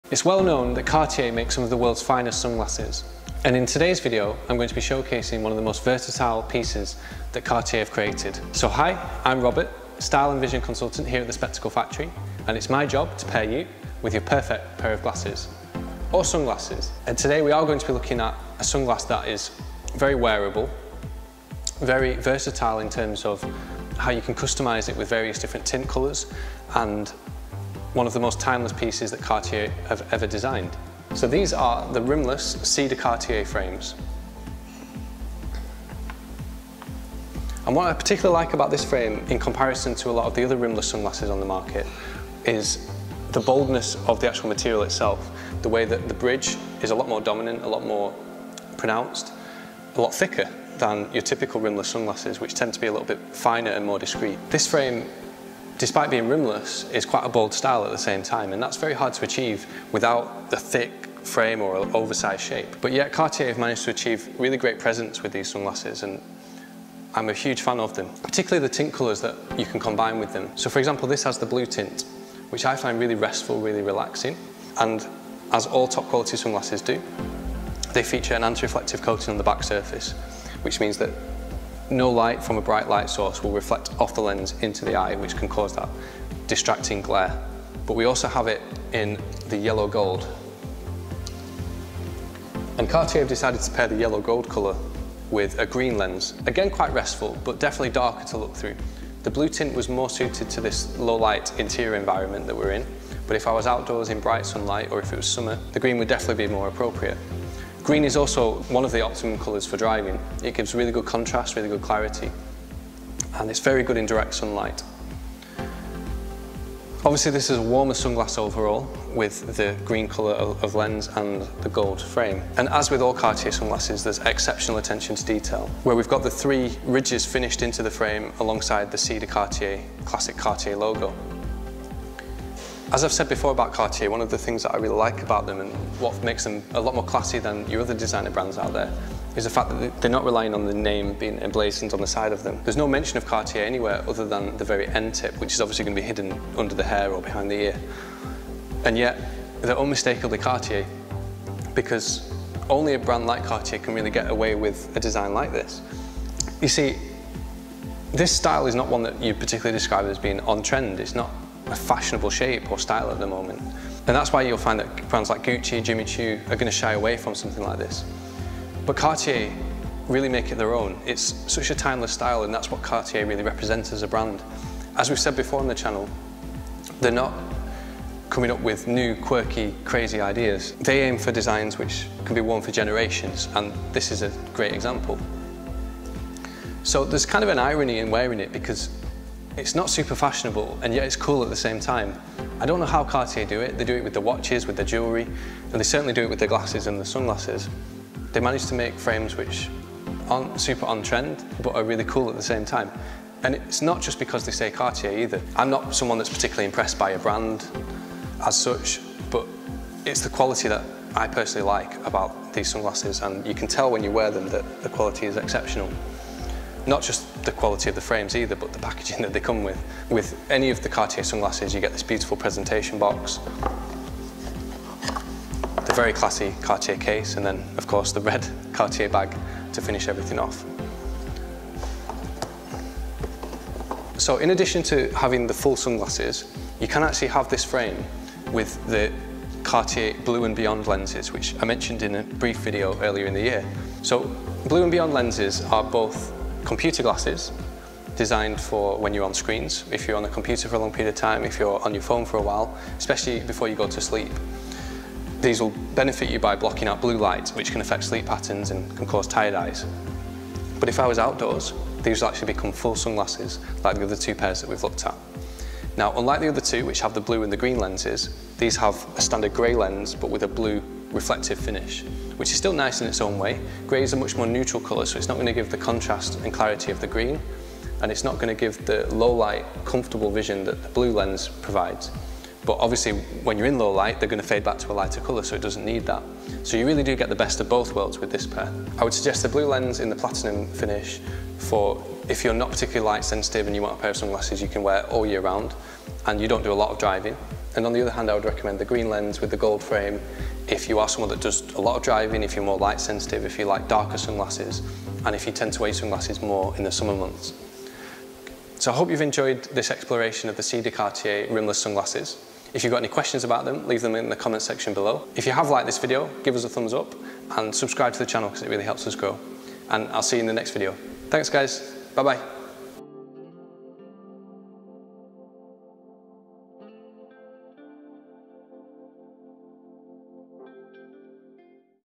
Cartier Rimless Sunglasses Showcase | C de Cartier Frames
The most classic Cartier eyewear is the C de Cartier line. Enjoy as we showcase the most elegant take on the C de Cartier design featuring two beautiful colour combinations.